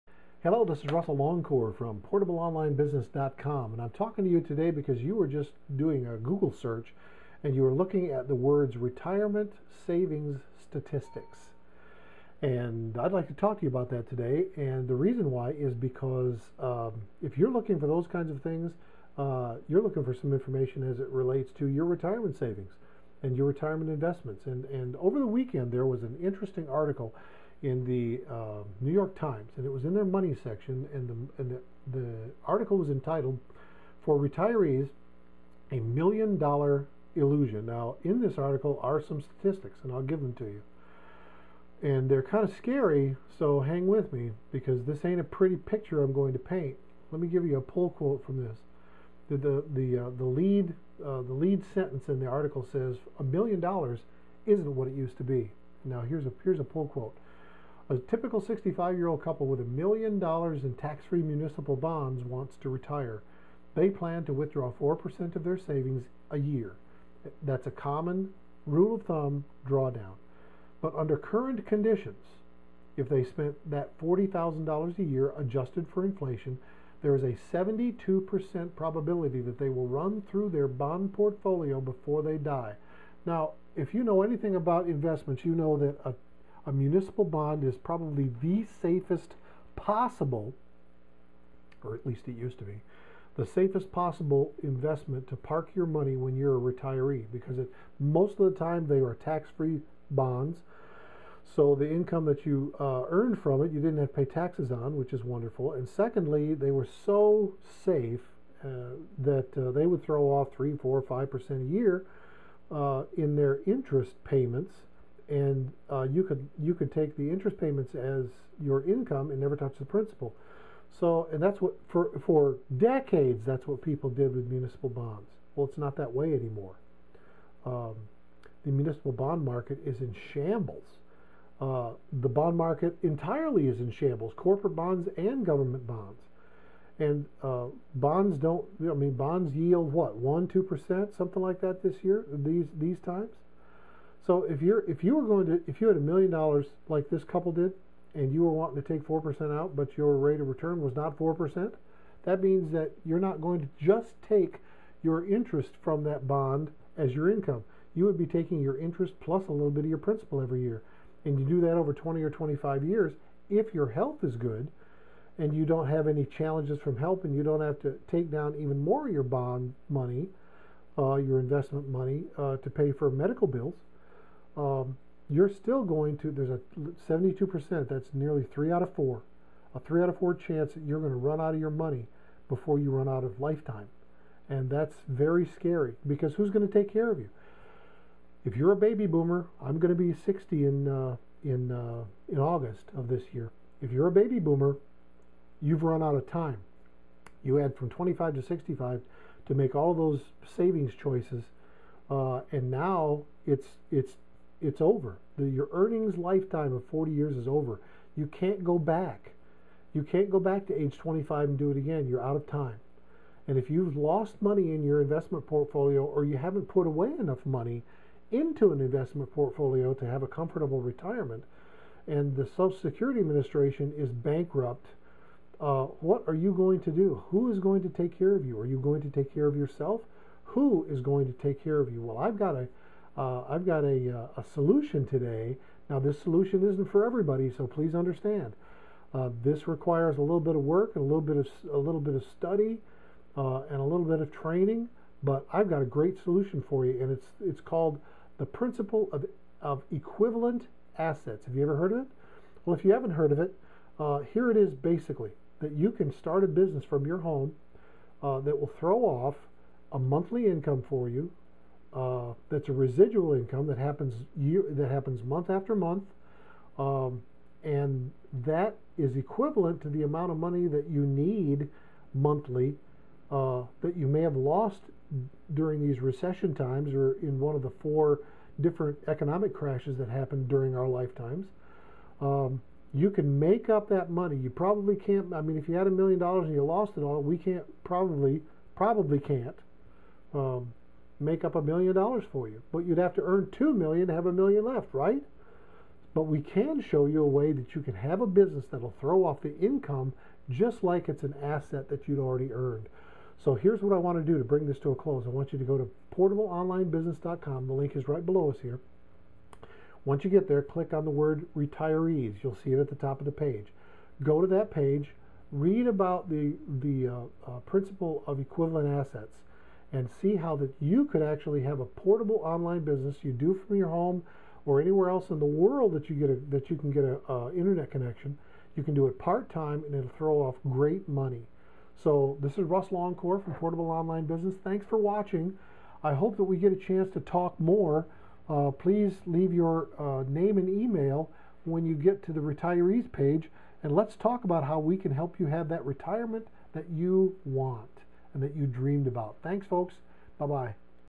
Retirement Savings Statistics: Don't Be A Statistic
Retirement savings statistics are brutal in today's economy. The New York Times article below states that there is a 72% probability that you will outlive your investments...even if you start out with $1 million dollars. Woman can expect to live to age 86, men to age 84. And 10% of those age 65 now will live past 95.

Retirement Savings are extremely vital for Baby Boomers. And in today's economy, it's difficult to find a safe investment that has good yields. The earning years of the Baby Boomers are mostly over. So,what do you do if your retirement savings have lost value? Who is going to care for you if the money runs out before you die?

See how the Principal of Equivalent Assets can provide a steady, reliable retirement income for you...even if you haven't saved ANY retirement investments yet. A home business should be part of your retirement income plans.